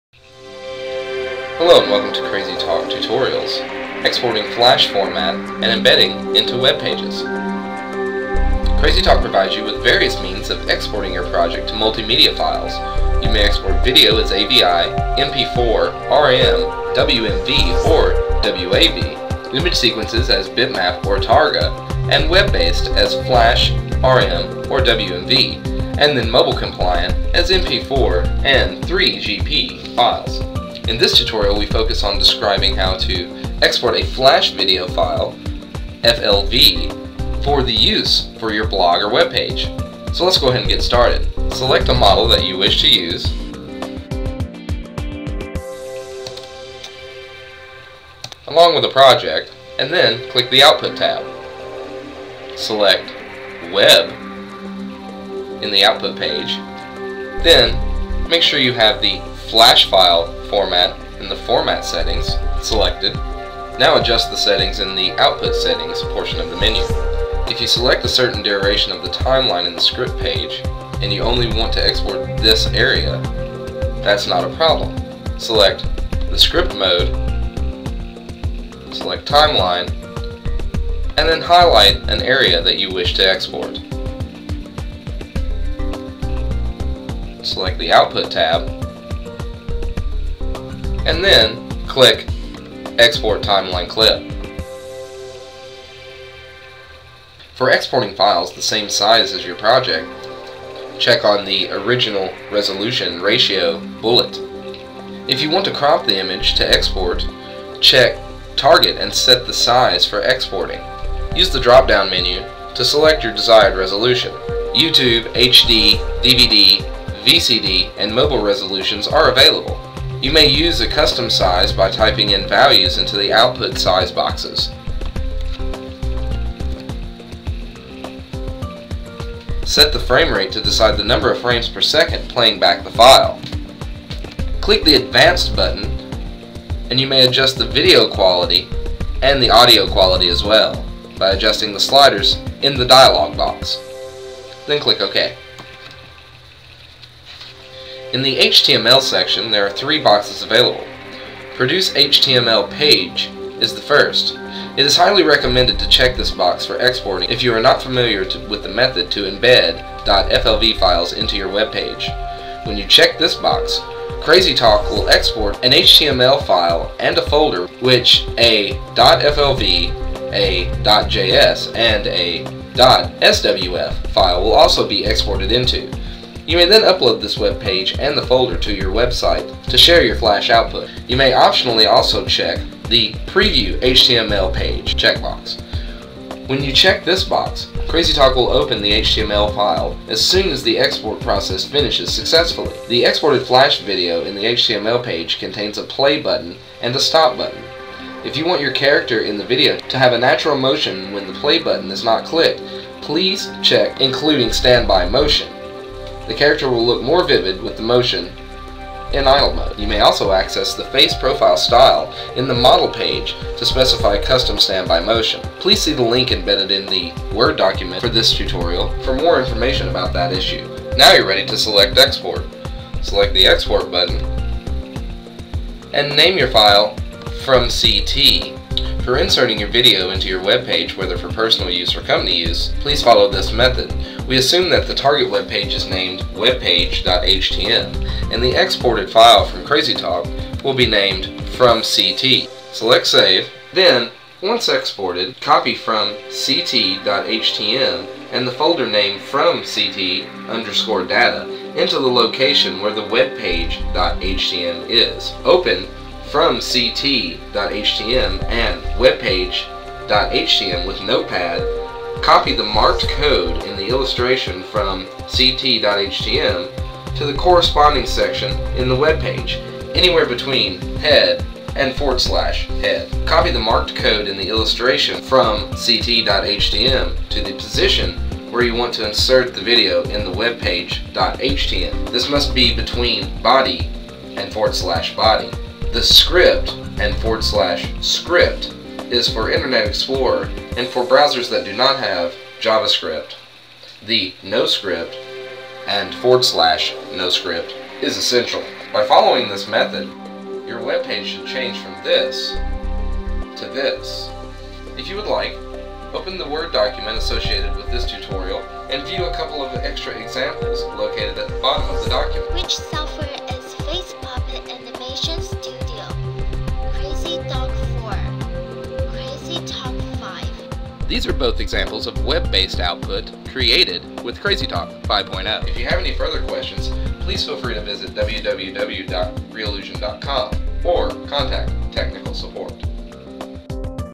CrazyTalk5 Tutorial - Exporting and Embedding Flash Format
This tutorial describes how you can export Flash files and embed them into your web pages for personal or commercial purposes.